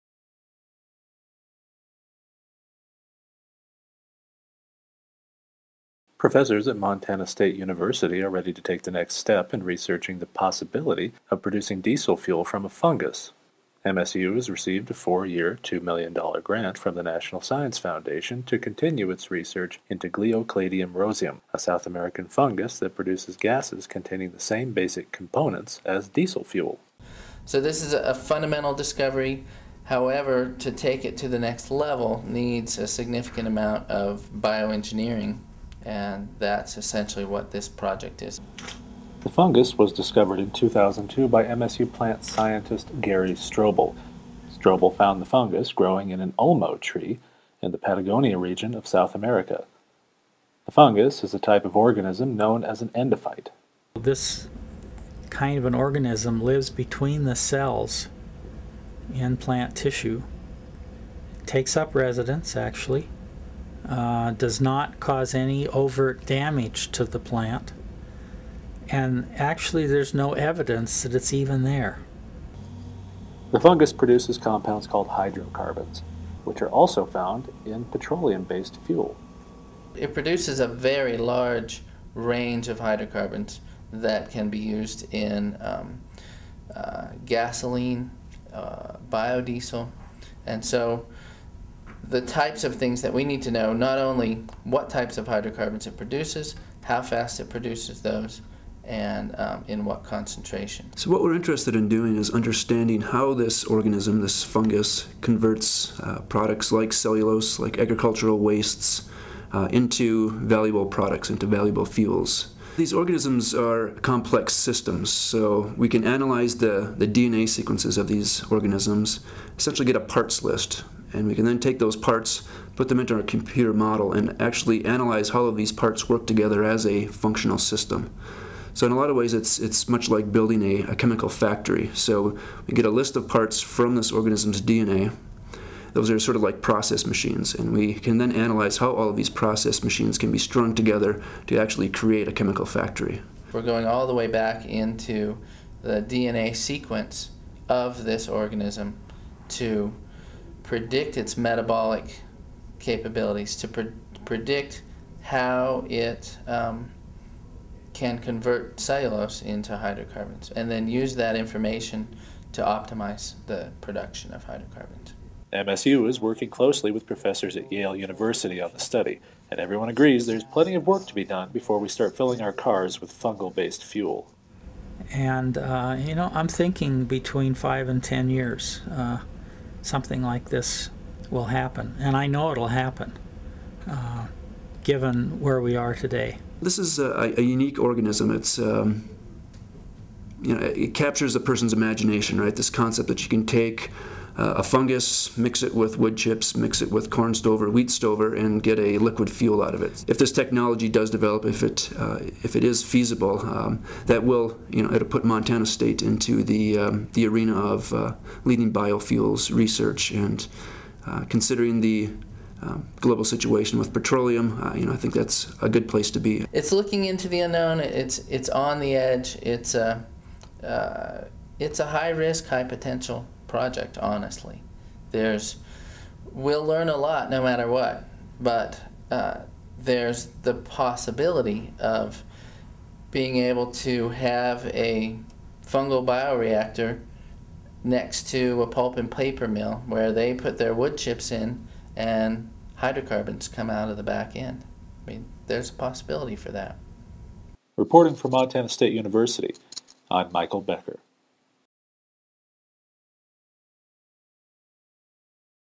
Montana State Researchers Receive $2M NSF Grant to Continue Fungal Biofuel Research
Montana State University professors are taking the next step in research that could make it possible to produce biofuel from wood chips using a fungus discovered in South America.